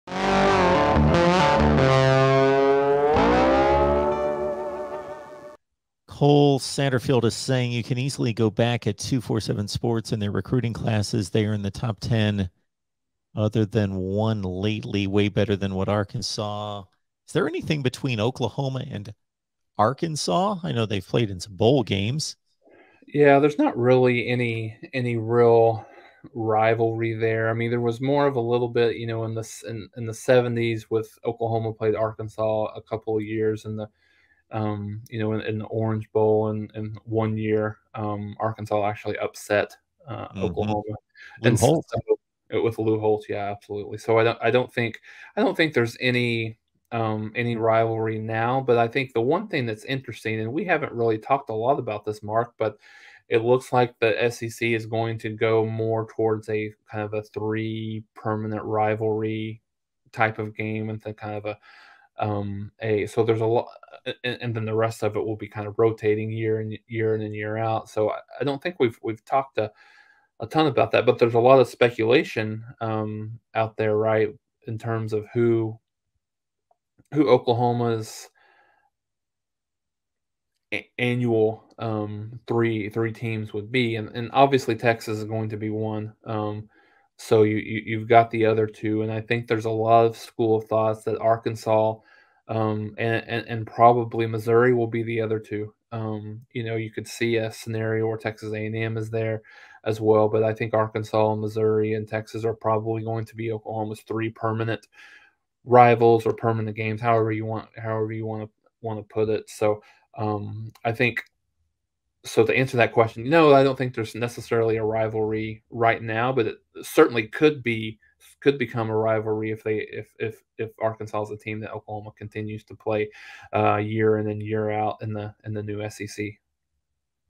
Will OU Start a New Rivalry With Arkansas When They Join the SEC?
With Oklahoma joining the SEC, will Oklahoma revive their one-time rivalry with the Razorbacks as one of the three permanent conference teams along with Texas and Missouri?